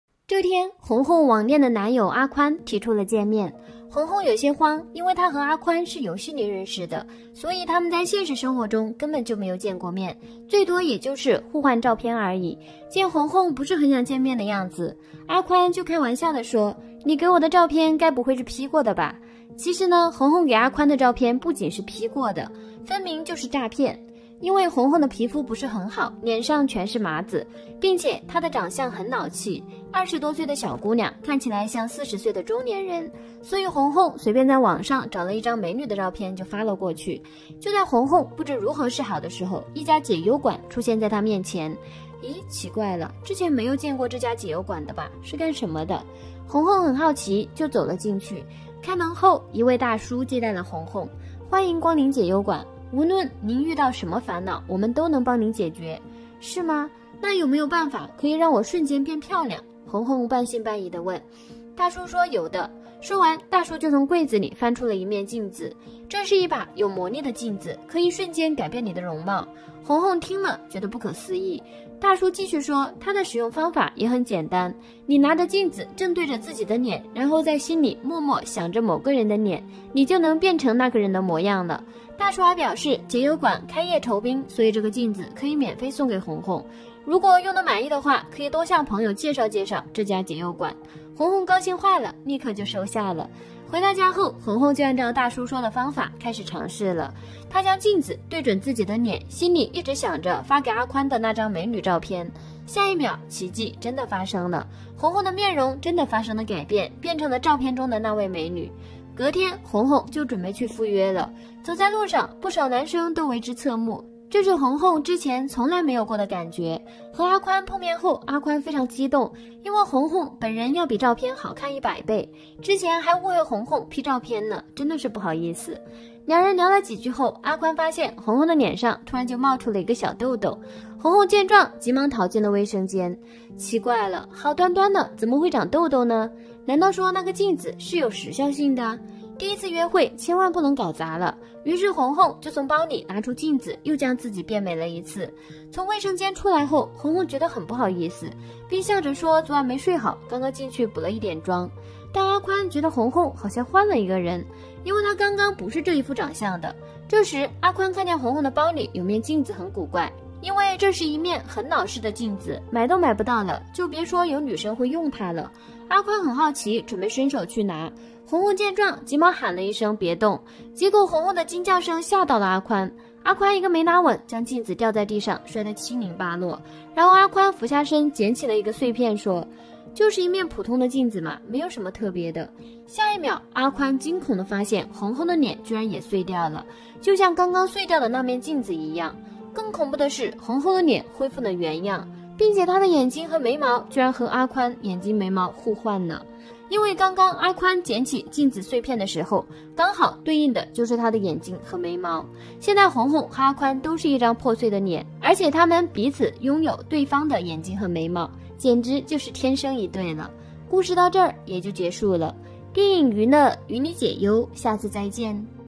丑女为了和男网友见面，用魔镜强行给自己美颜，最后却发生了意外！
惊悚漫画《魔镜美颜》